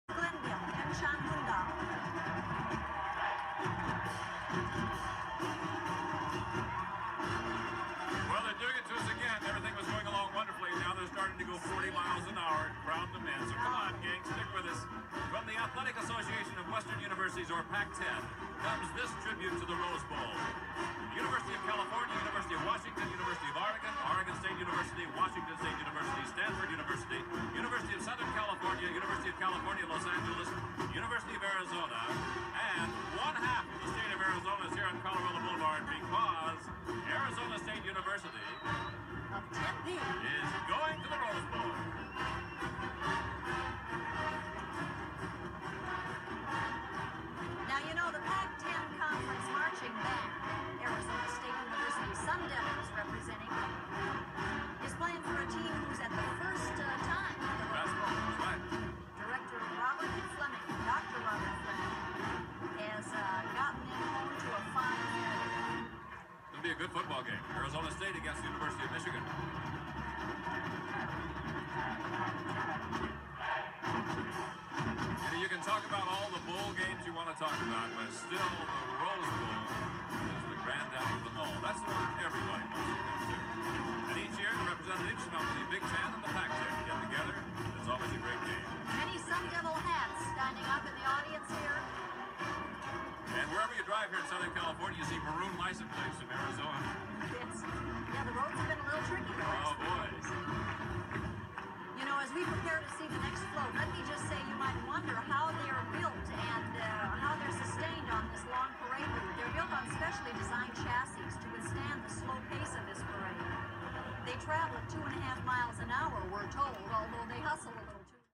Arizona State University Sun Devils 1st Rose Parade entry 1987
In 1986, the ASU Sun Devil football team won its first Pac 10 title, thus earning a New Years Day Rose Bowl berth against Big 10 champs University of Michigan Wolverines.  This was the actual television footage of the ASU Marching Band and Cheerleading Squad in the Tournament of Roses parade that day.  I make no claim of ownership to the copyright of any enclosed material.  This is purely for the enjoyment of Sun Devils everywhere.